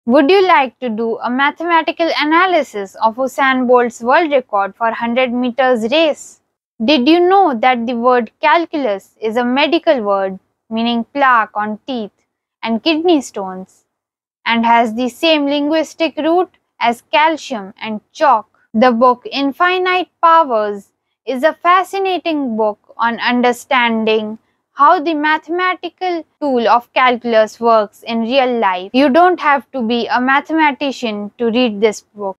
Infinite Powers | Book Recommendation
Link to book:  Infinite Powers by Steven H. Strogatz
(A) Paperback:

(B) eBook Kindle

Calculus, a branch of mathematics, is concerned with the study of change and motion. It provides powerful tools for understanding the behavior of functions and their rates of change. At its core, calculus comprises two main concepts: differentiation and integration.